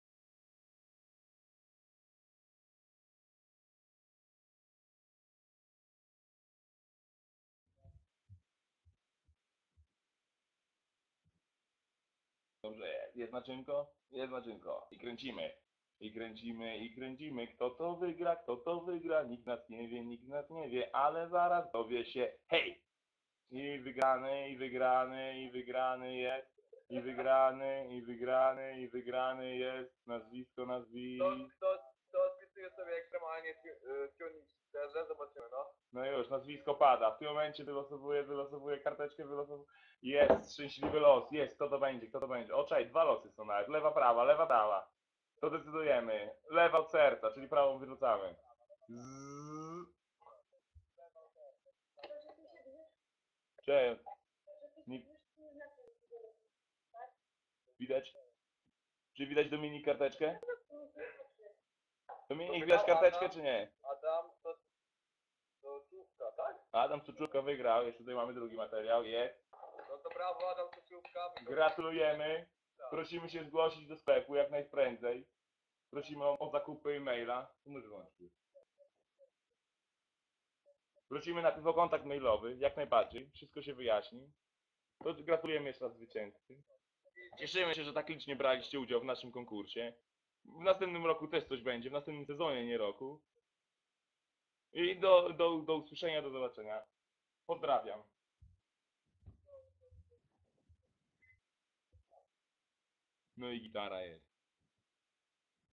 Wyniki konkursu "zrób zakupy w S-X-T i wygraj 2000zł na tuning"
Jedyny taki konkurs na Polskiej scenie motoryzacji !!! Tylko w S-X-T ;)